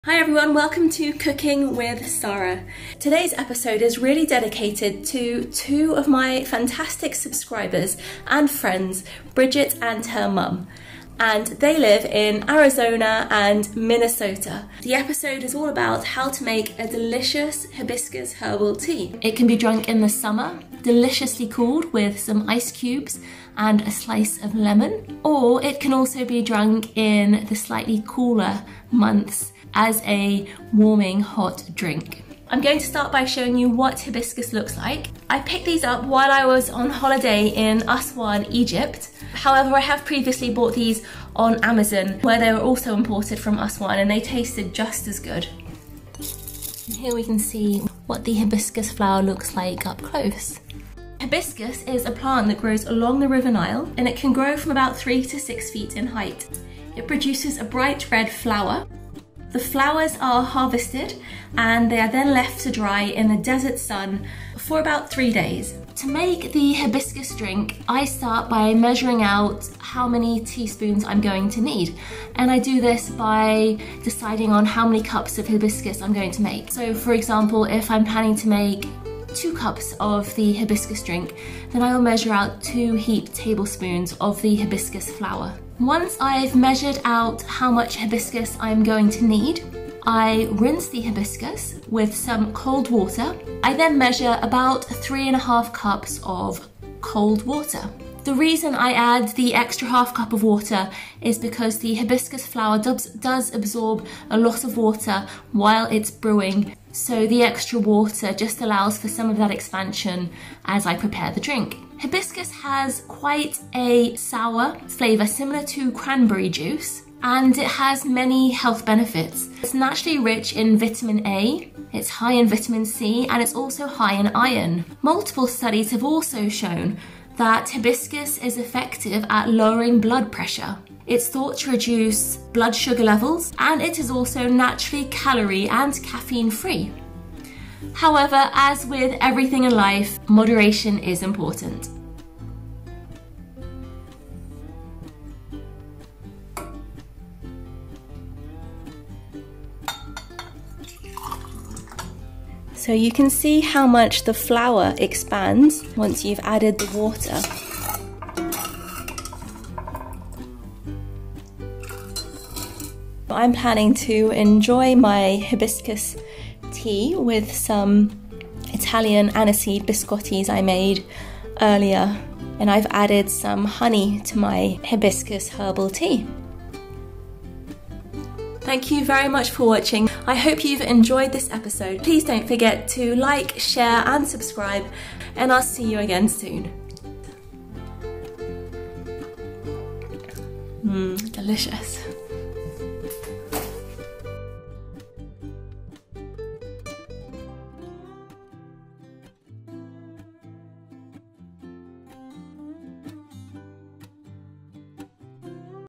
Delicious Herbal Weight Loss Tea! How to Make Hibiscus Tea + Health Benefits!
Are you looking for an alternative healthy, nutritional, herbal tea drink to help you in your diet and to lose weight?

Everything about how to make a delicious herbal hibiscus tea drink. This is a drink that I have drunk for as long as I can remember, it can be drunk in the summer deliciously cooled with ice and a slice of lemon, or as a hot warming  drink in cooler months.

I picked up this hibiscus while I was on holiday in Aswan, Egypt, however I have previously bought hibiscus from amazon (that had been imported form Aswan) and it was also delicious.

It grows along the Nile on a plant that grows 3-6 feet in height. Once it is harvested the flowers are laid out to dry under the desert sun for about 3 days .

To make a hibiscus drink I start by measuring out how much I need – usually 1 tablespoon for cup of drink I plant to make. I then rinse the hibiscus in cold water and add 3 and a half cups of water – the extra water is to allow for the dried plant absorbing some water as it expands.

Hibiscus has many health benefits – it is naturally rich in vitamin A, vitamin C and iron.

Multiple  studies on hibiscus have shown that it is effective at lowering blood pressure. It is thought to reduce blood sugar levels and It is also naturally calorie and caffeine free. However as with everything in life, moderation is important.

You add the hibiscus into water and leave it to boil for about 5 minutes, the longer you leave it the darker it will get.

It can be served hot or cold and honey or sugar can be assed to sweeten as desired.